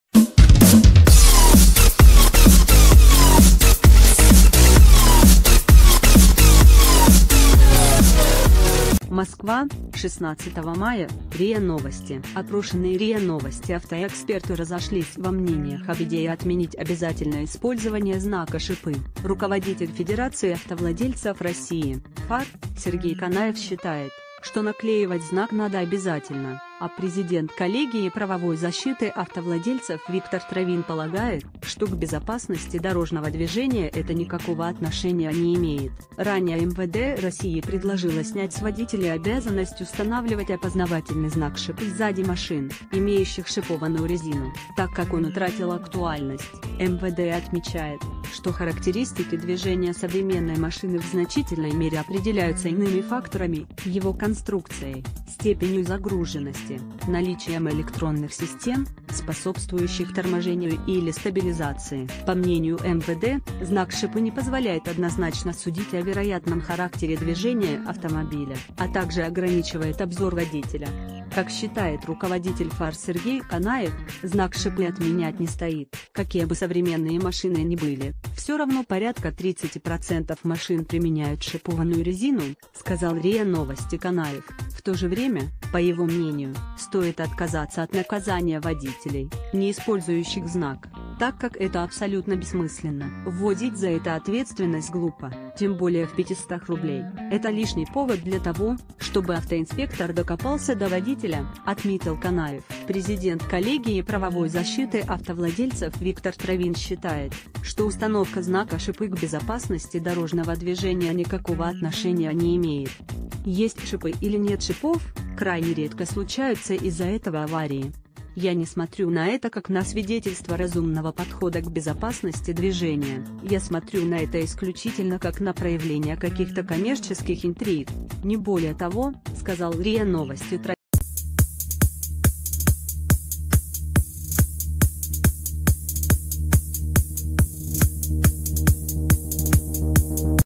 Эксперты поделились идеей отмены использования знака «Шипы»,
МОСКВА, 16 мая — РИА Новости. Опрошенные РИА Новости автоэксперты разошлись во мнениях об идее отмен...